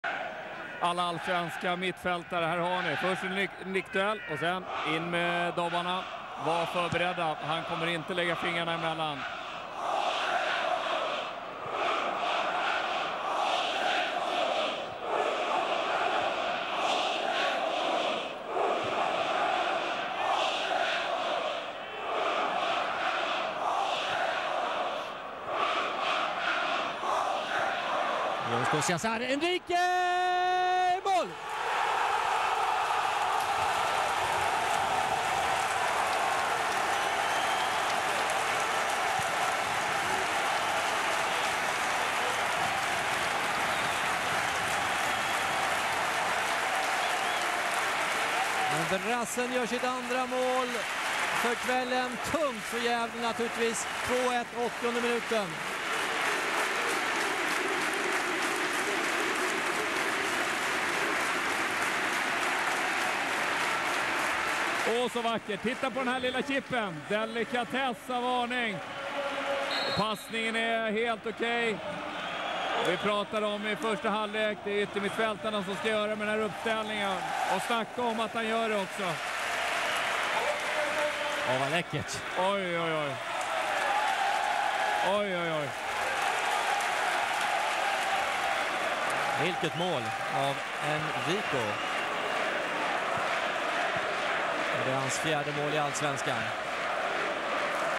Enrico Cardoso Nazaré scores with a wonderful chip
Enrico Cardoso Nazaré scores with a wonderful chip. Allsvenskans nicest goal this year?